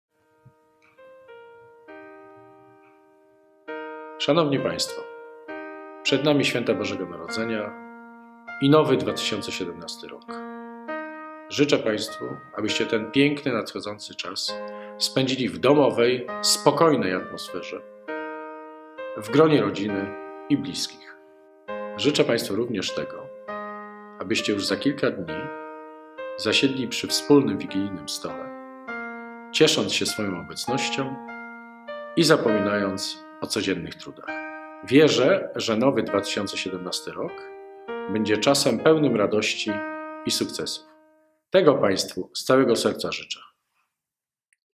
Zbigniew Hoffmann - życzenia z okazji Bożego Narodzenia i Nowego Roku
Życzenia wojewody wielkopolskiego Zbigniewa Hoffmanna | Boże Narodzenie i Nowy 2017 Rok